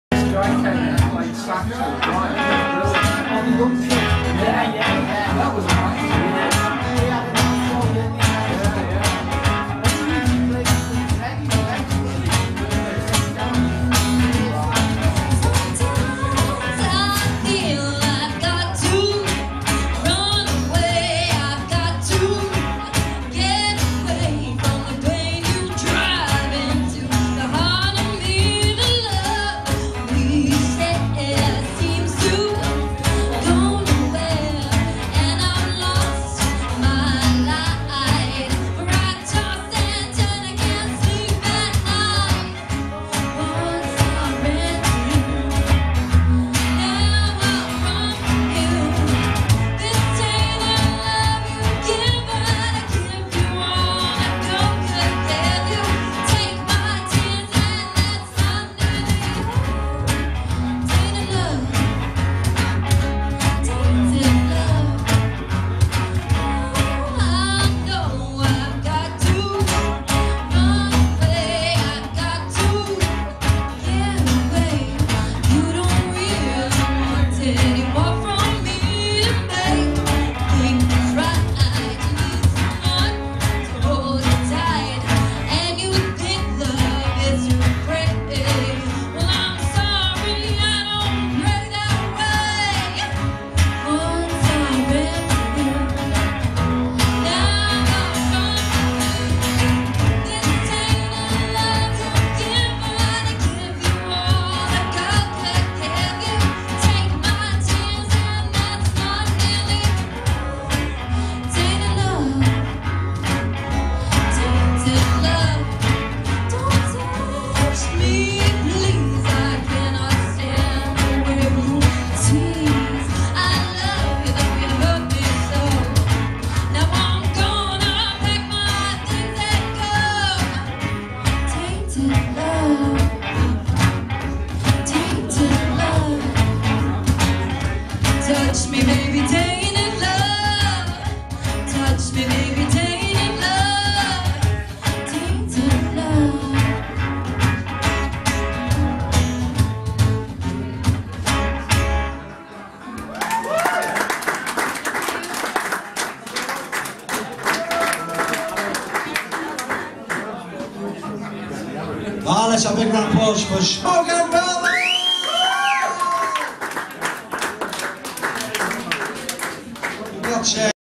smoking velvet
my lads band with me playing the cjon  at rainford junction uk "smoking velvet"